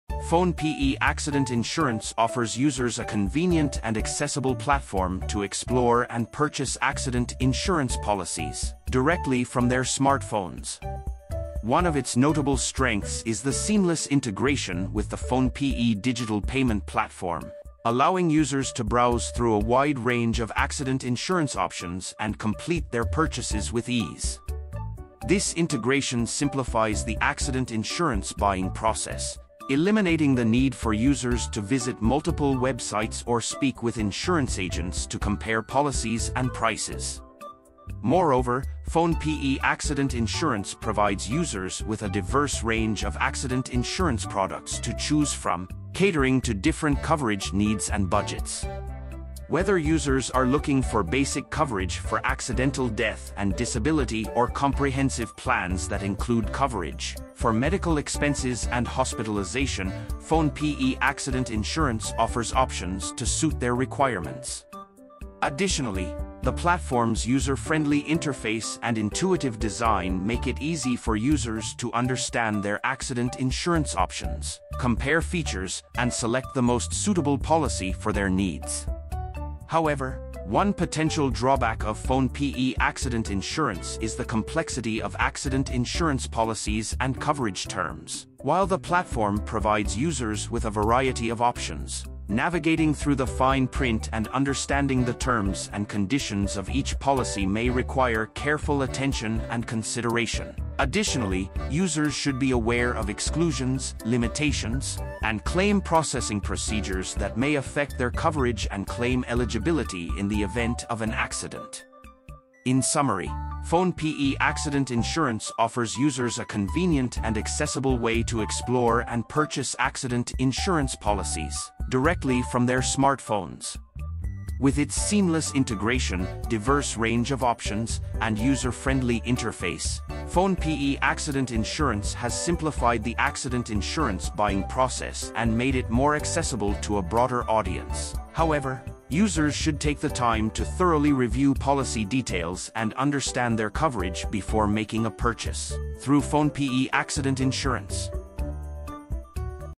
🔥 PhonePe Accident Insurance Review: Affordable Protection with Some Considerations
PhonePe Accident Insurance offers a straightforward and affordable option for individuals looking to secure financial protection in the event of an accident. One of the standout features of this insurance is its low premium rates, making it an accessible choice for a wide range of users. The coverage provides protection in case of accidental injuries, hospitalization, and even death, offering peace of mind to policyholders who are concerned about the financial impact of unforeseen accidents. Additionally, the policy is easy to purchase and manage through the PhonePe app, which adds to the convenience of securing coverage on the go.

Another advantage of PhonePe Accident Insurance is its simplicity and transparency. The app provides clear details on what is covered, along with the premium rates, allowing users to easily understand the terms of their policy before committing. The claims process is generally quick and straightforward, with the app enabling users to file claims digitally, reducing the need for cumbersome paperwork. This digital-first approach streamlines the experience, making it a great choice for those who prefer managing their insurance through an easy-to-use mobile platform.

However, there are some drawbacks to consider with PhonePe Accident Insurance. While the policy offers basic coverage for accidental injuries and hospitalization, it may not provide the extensive protection that more comprehensive health or life insurance policies offer. The coverage limits may be relatively low compared to more traditional accident insurance providers, which could be a concern for individuals seeking higher coverage amounts or more extensive benefits. Additionally, while the app’s user interface is generally easy to navigate, some users have reported occasional issues with processing claims or getting timely customer support during peak periods.

In conclusion, PhonePe Accident Insurance provides an affordable and accessible way for individuals to protect themselves in the event of an accident. Its low premiums, simplicity, and quick claims process make it a solid option for basic coverage. However, the limited coverage and potential delays in claims processing may make it less suitable for those seeking more extensive protection or needing faster support. Overall, PhonePe Accident Insurance is a great choice for those looking for budget-friendly, no-frills accident coverage, but it may not fully meet the needs of everyone, especially those with more complex insurance requirements.